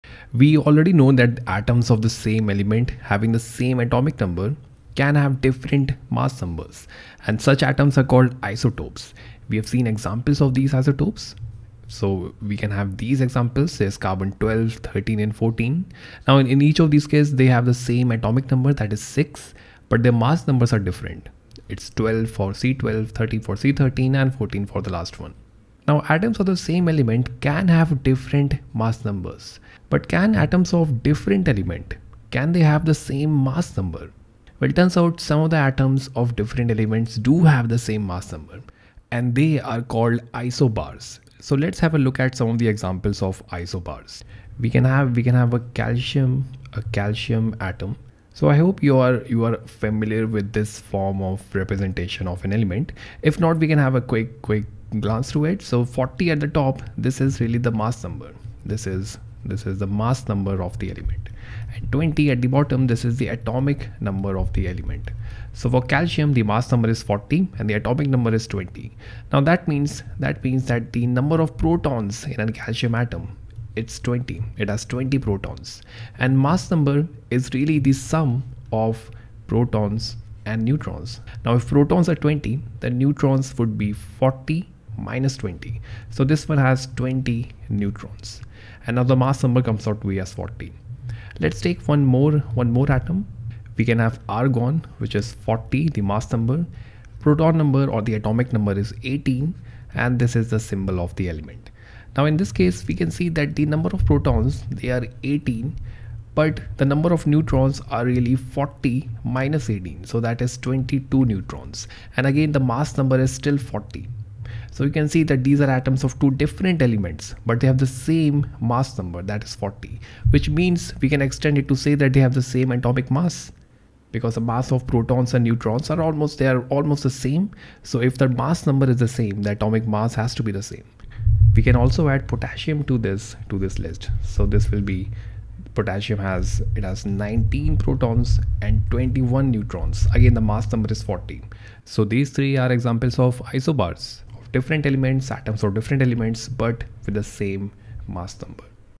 Isobars | Structure of an atom | Chemistry | Khan Academy
We've seen atoms of same elements with same atomic number but different atomic masses, but now, let's have a look at atoms of different elements with same atomic mass!